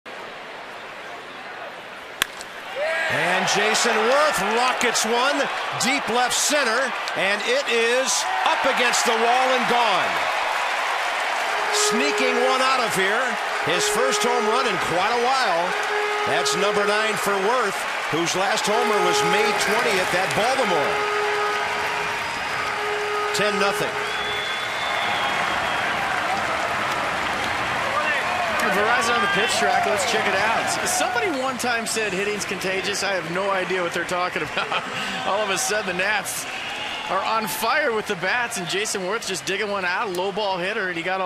2011/06/15 Werth goes yard
STL@WSH: Werth takes Tallet deep in the eighth